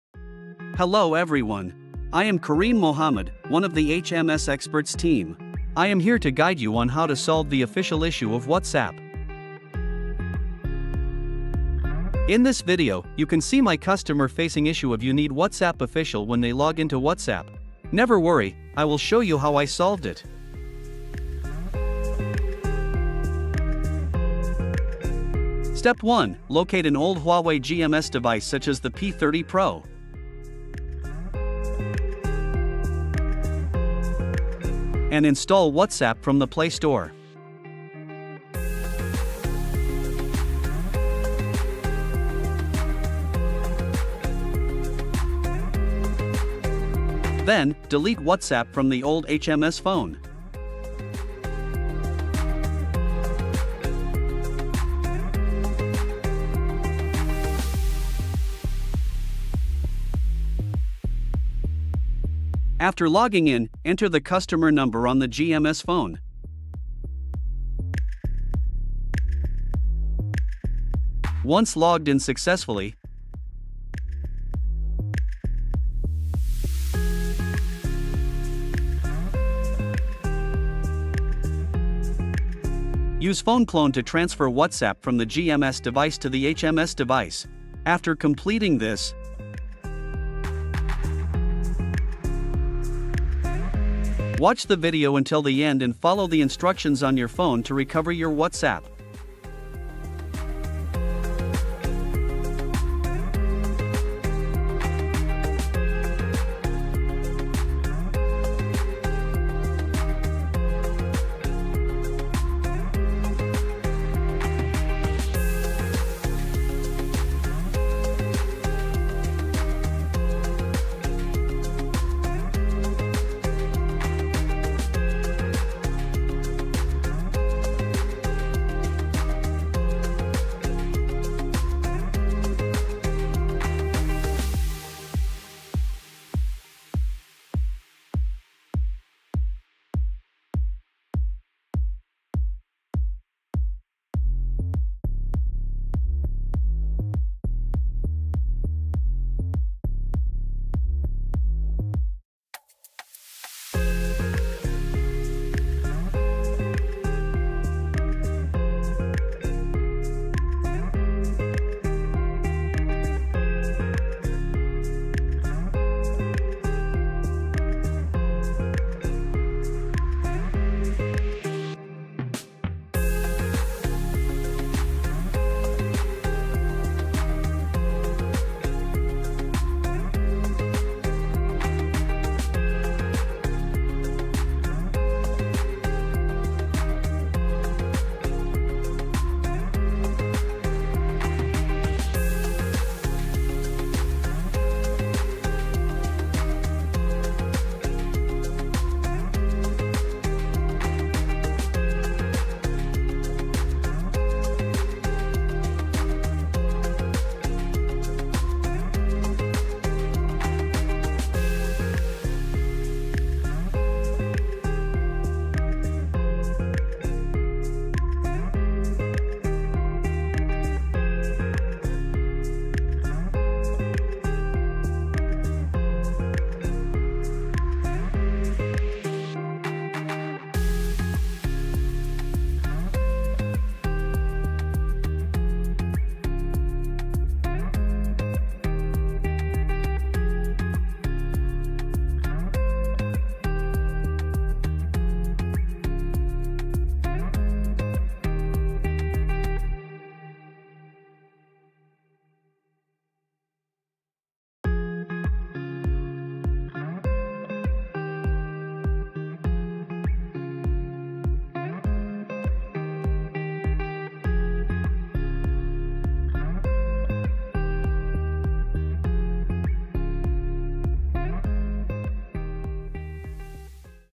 How to Fix the WhatsApp Official Issue in Simple Steps
Are you facing issues with the official WhatsApp app? Don't worry; I've got you covered! In this video, I'll walk you through a straightforward process to resolve the problem and get your WhatsApp running smoothly again. Just follow the steps, and you'll be back on track in no time!

Watch now and solve your WhatsApp issue with ease!